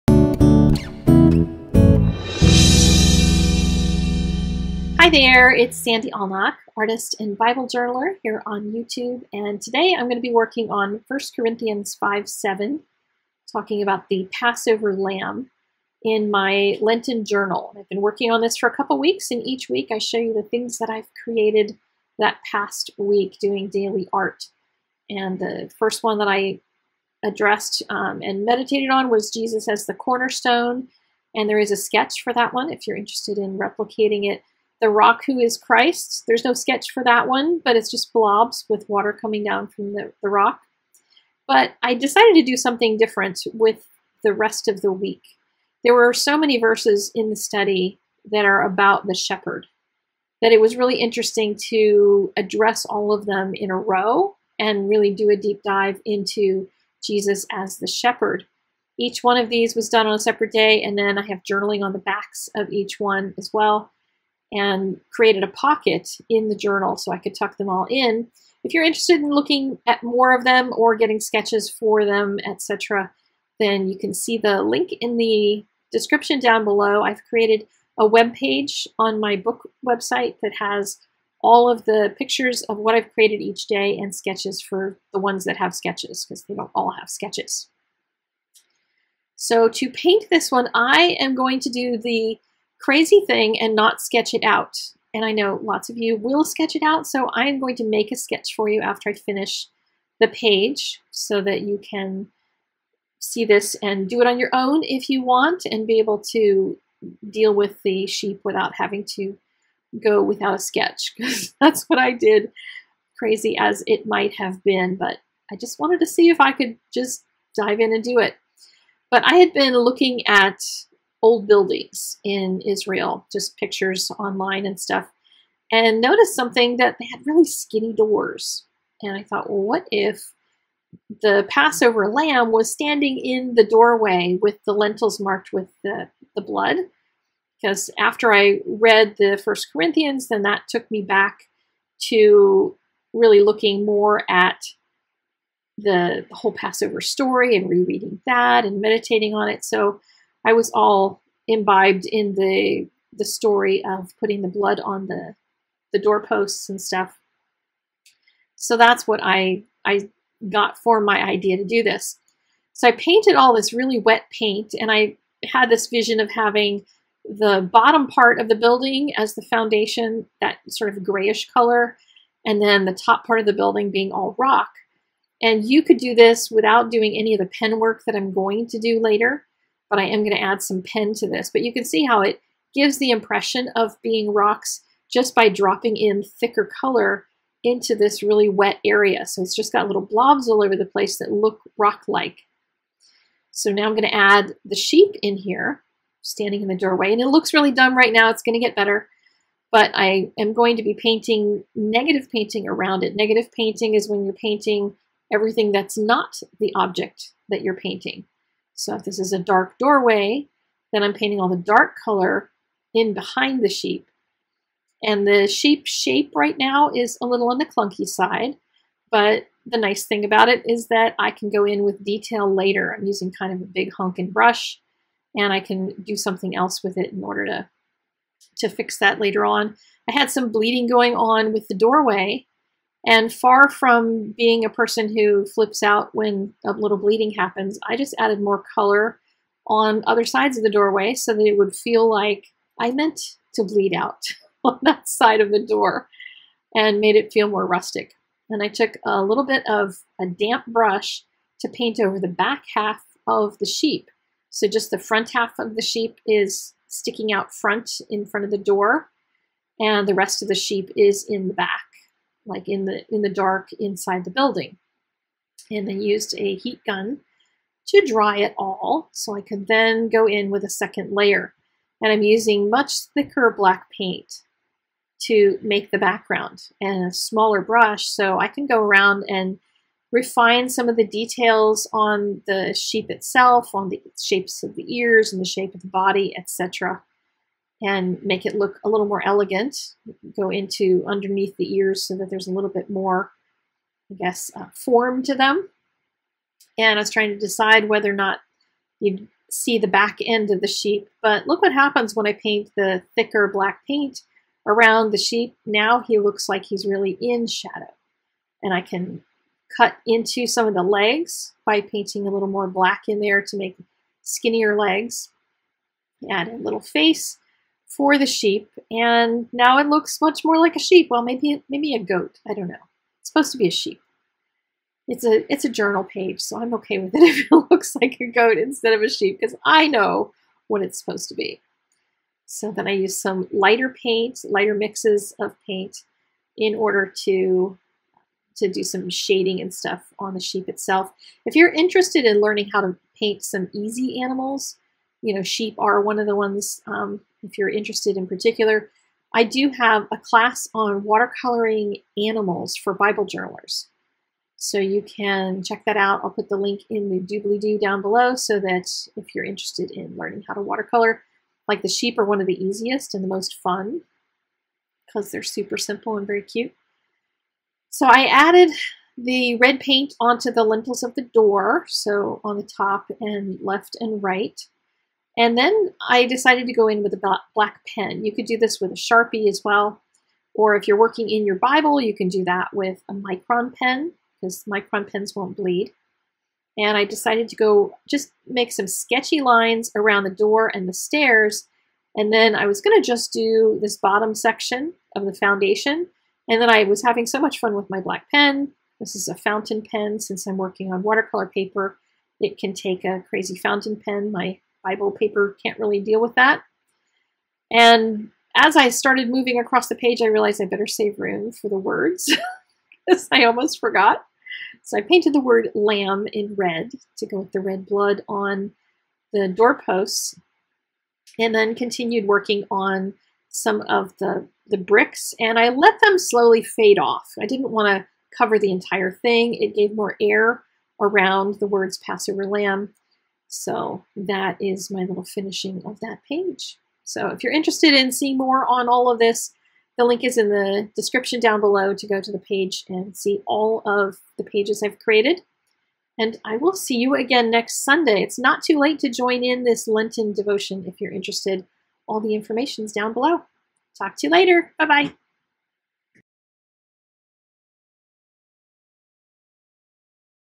Lenten Journal Day 19: Passover Lamb (1 Cor 5:7)
Another in my journal pages! We're on Day 19 if you're keeping up - but you can join in anytime to aid your heart-preparation for Easter. No pressure - just study, pray, and create!

"Get rid of the old yeast, so that you may be a new unleavened batch—as you really are. For Christ, our Passover lamb, has been sacrificed." 1 Cor 5:7

MISC
Tomoe River Paper